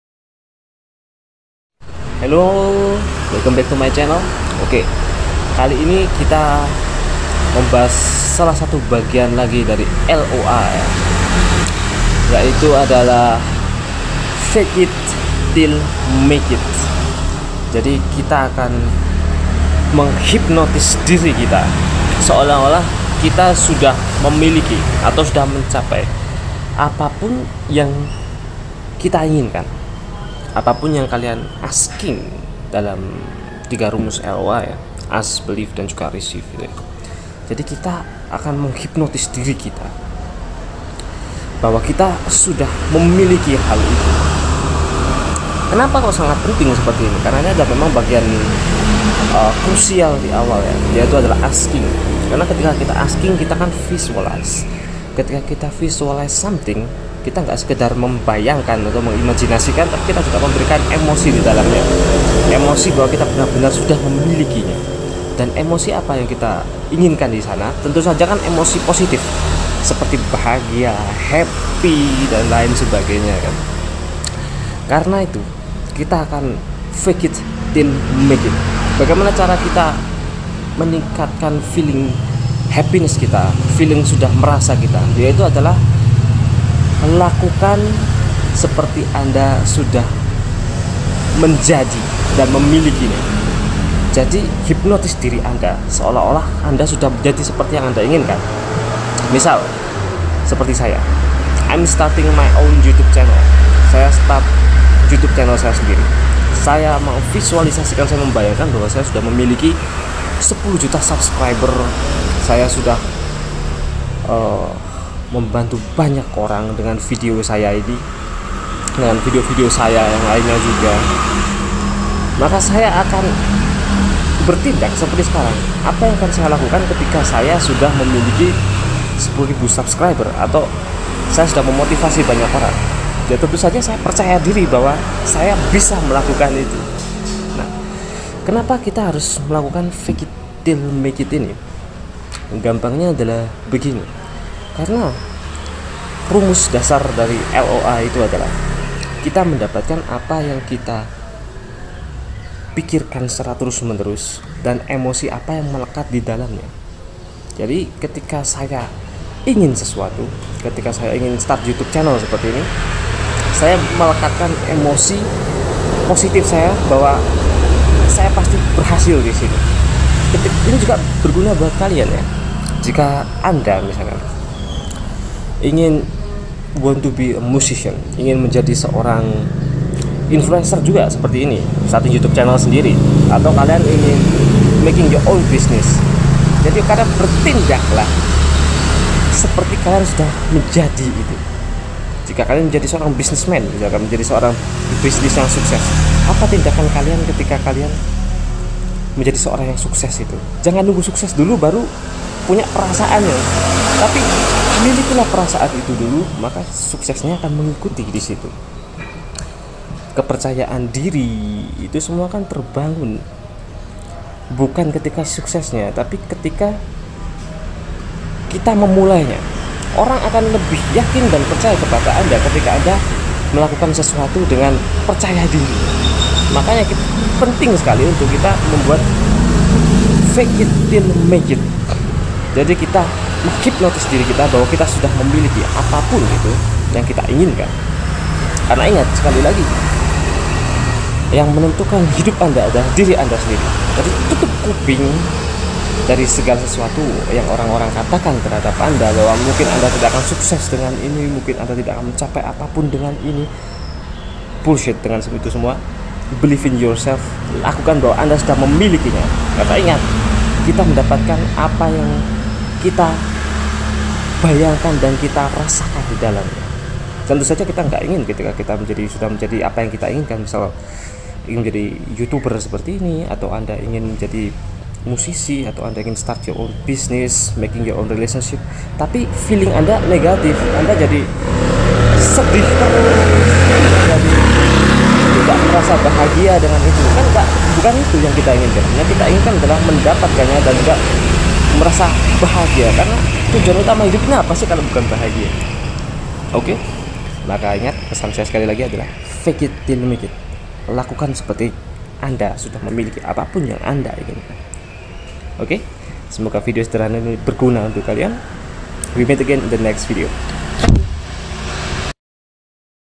FAKE IT TILL MAKE IT!!!Berpikir dan Bertindak Seperti Sudah Memilikinya(Law of Attraction Indonesia)
'Feeling Having It Now' atau merasa sudah memiliki atau sudah menjadi apa yang anda inginkan adalah kunci untuk mensinkronkan vibrasi anda dengan SEMESTA...

Just do it !!! Saya yakin anda bisa !!!